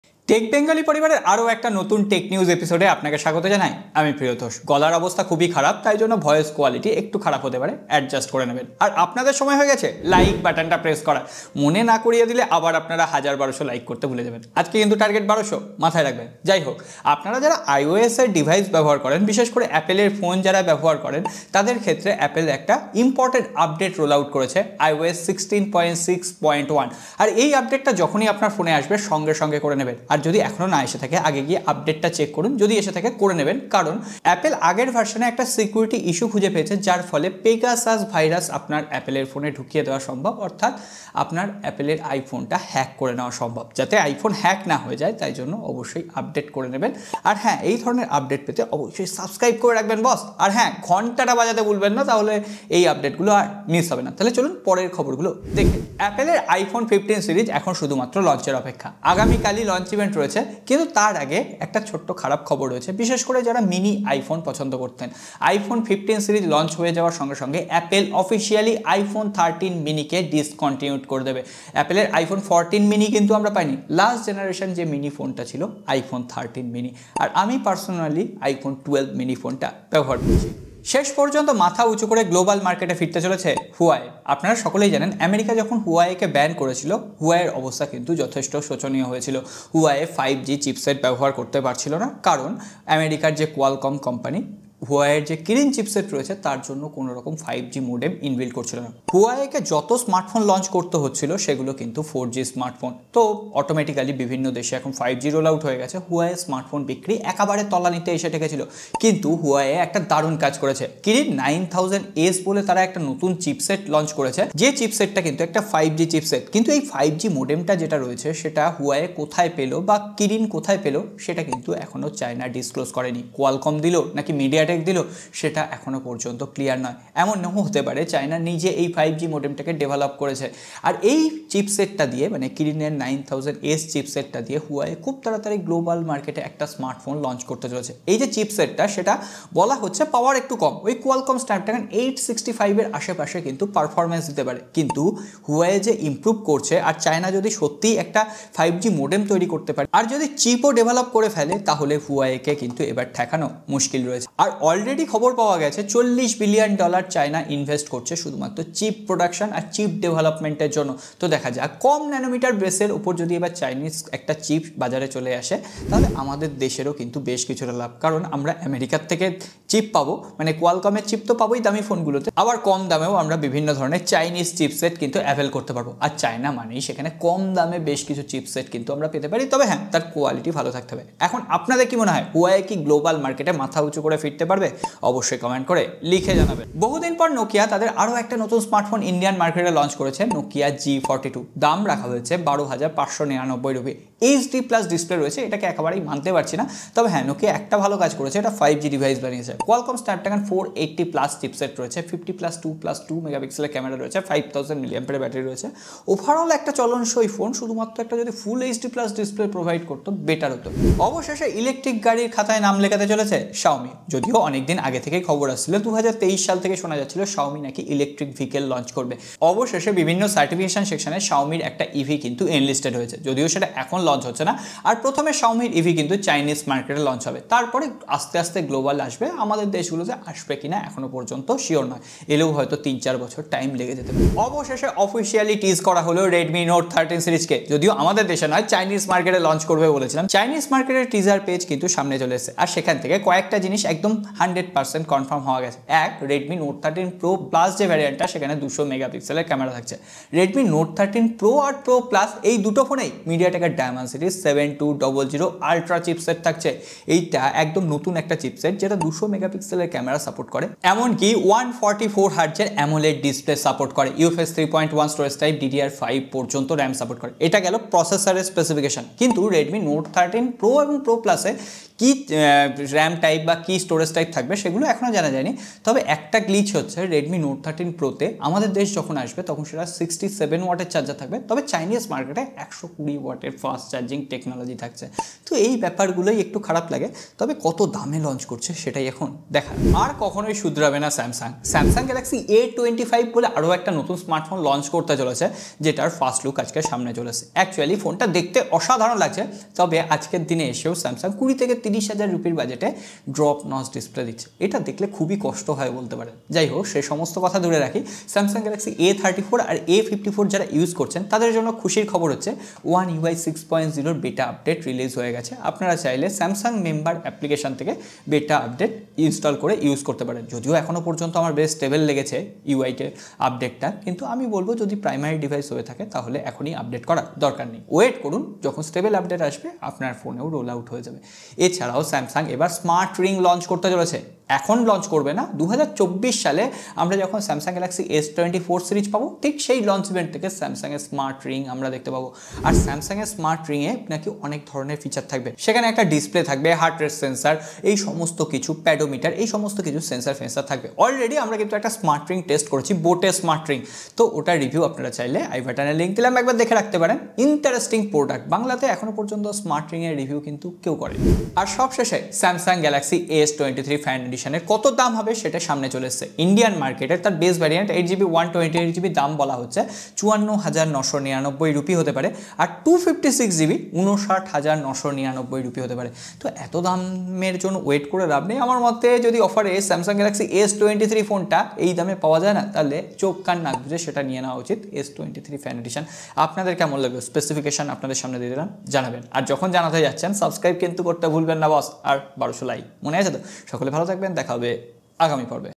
Samusng S23 FE কনফার্ম দাম 🔥 | Redmi Note 13 ফাটাফাটি ❤️ | iPhone user বিপদে ❗| Huawei ফিরছে 💪
Amazon Store
00:00 : Intro